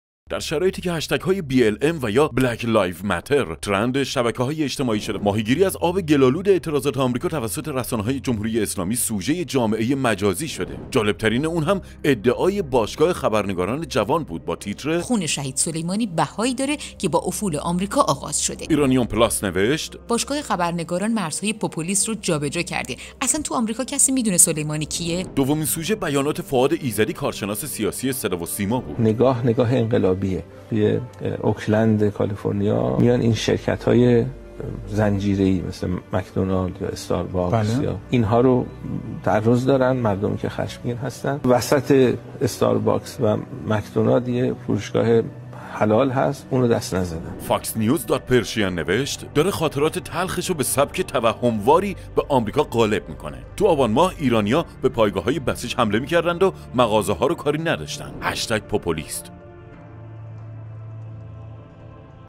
ماهیگیری از آب گل آلود آمریکا
با این بیانات مسئولین ایرانی، تن جرج فلوید تو‌ی گور نمی‌لرزه؟ تا این از خیال بافی مگه می‌شه؟ مگه داریم؟